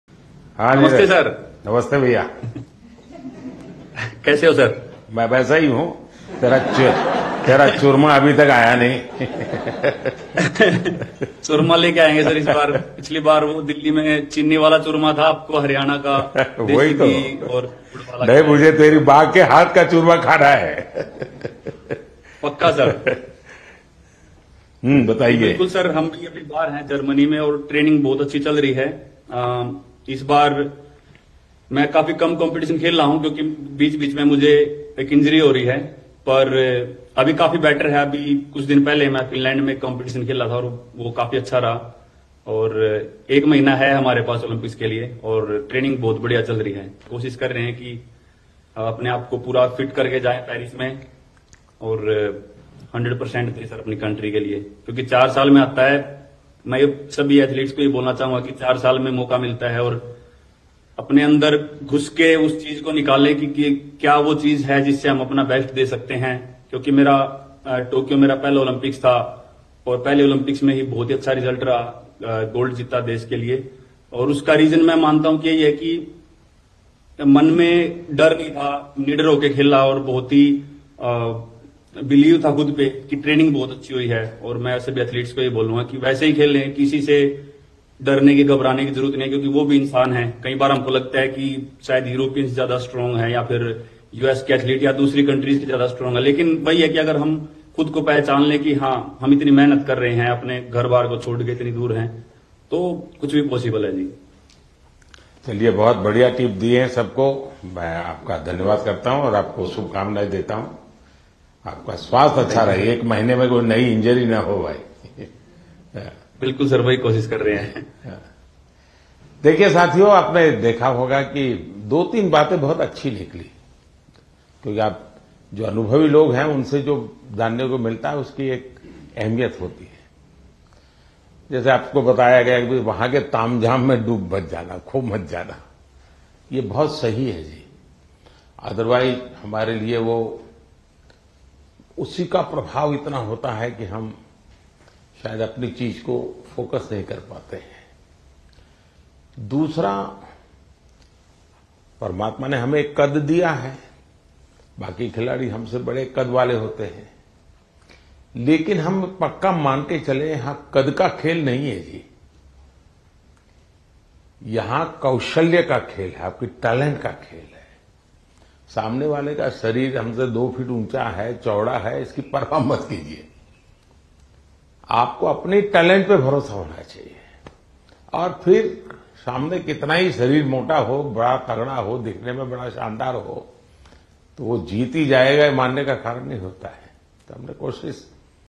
Paris Olympics Players से PM Modi की खास बातचीत, सुनिए Neeraj Chopra से क्या कहा?
PM Modi Meets Paris Olympics Players: प्रधानमंत्री नरेंद्र मोदी(PM MOdi) ने जुलाई महीने के अंतिम सप्ताह में आरंभ हो पेरिस ओलंपिक (Paris Olympics)में भारतीय खिलाड़ी के उत्कृष्ट प्रदर्शन की उम्मीद जताते हुए भारतीय दल को शुभकामनाएं(Paris Olympics 2024 ) दीं और देशवासियों से खिलाड़ियों का उत्साह बढ़ाने का आग्रह किया.पीएम मोदी ने इस दौरान नीरज चोपड़ा(Neeraj Chopra) से एक खास चीज की मांग की. देखिए वीडियो

PM Modi Meets Paris Olympics Players, Olympics Players 2024, Paris Olympics 2024,PM MOdi Meet Players,Neeraj Chopra, PV Sindhu,Manu Bhakar